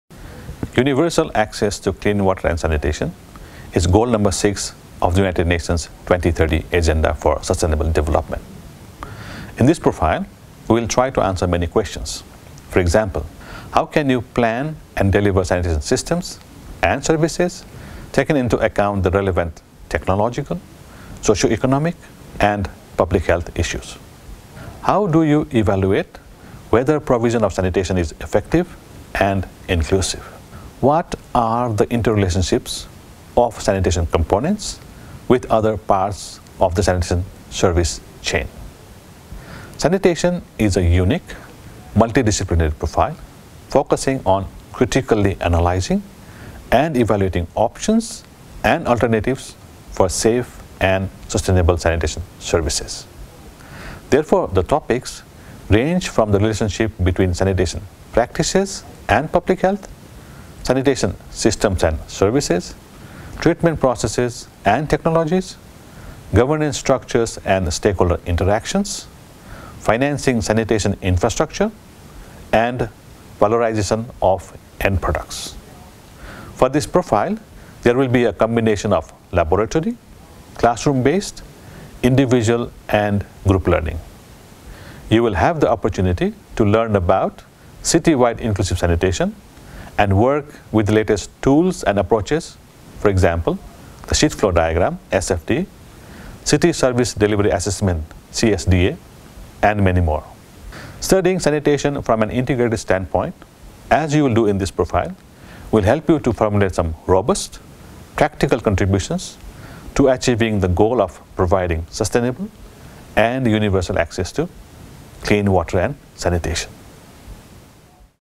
Sanitation Profile
Shirish Singh, Senior Lecturer/Researcher in Sanitation about Sanitation Profile of MSc in Water and Sustainable Development